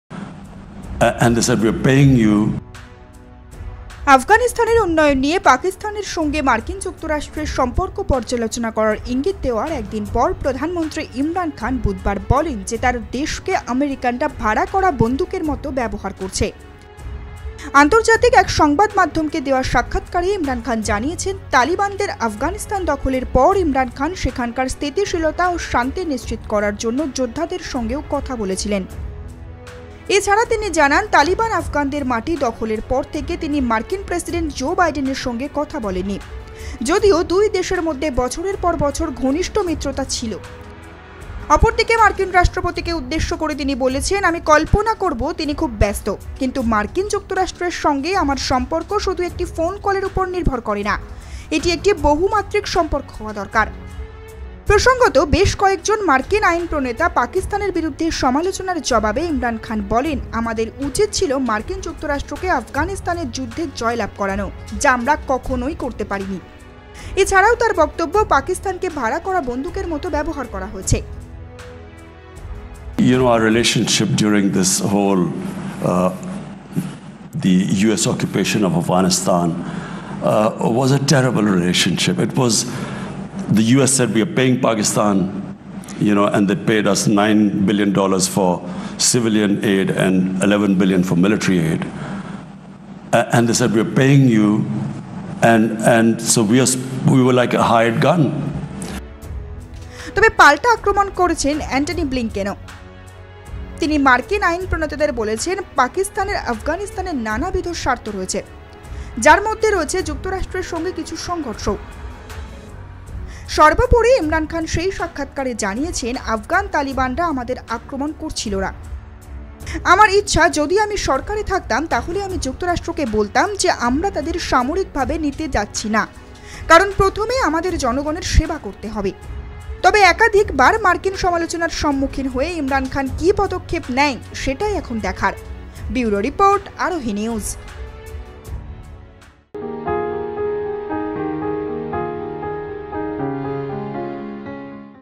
পাকিস্তানের প্রধানমন্ত্রী ইমরান খান আফগান সংকট নিয়ে মার্কিন সমালোচনার প্রতিক্রিয়া জানিয়েছেন
মার্কিন যুক্তরাষ্ট্রের নানা সমালোচনার জবাব দিলেন এবার পাক প্রধানমন্ত্রী ইমরান খান। তিনি জানিয়েছেন- "ভাড়া বন্দুকের মতো ব্যবহার করা হয়েছে পাকিস্তানের সঙ্গে।"